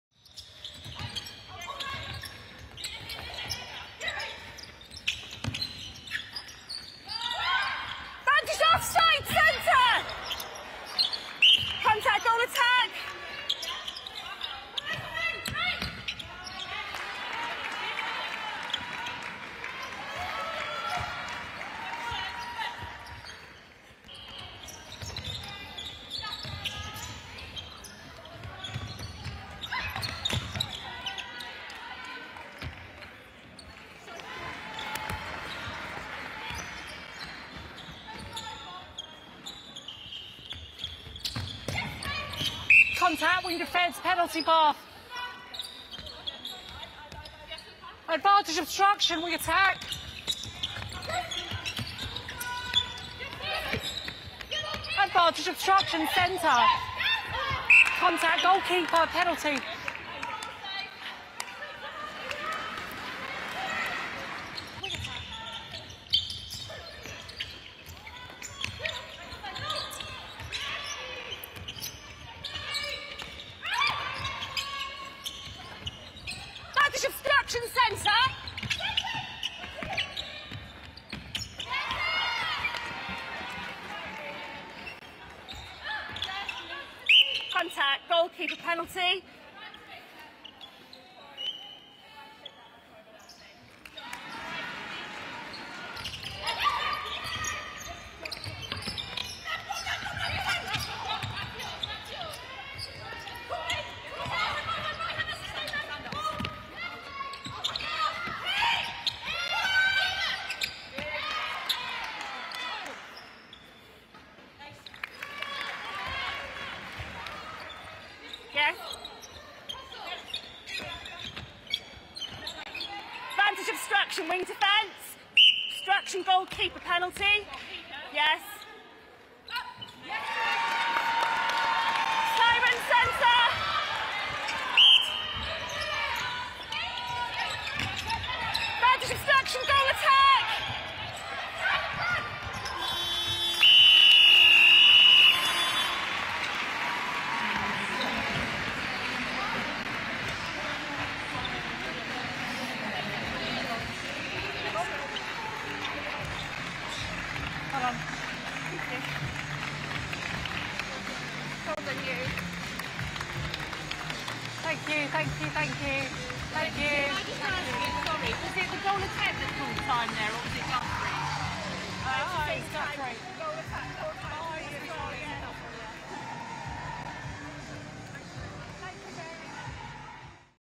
Highlights | Strathclyde Sirens 37-48 Team Bath Netball | VNSL 2021
Highlights of the Vitality Netball Superleague 2021 Round 17 / 18 fixture between Strathclyde Sirens and Team Bath Netball.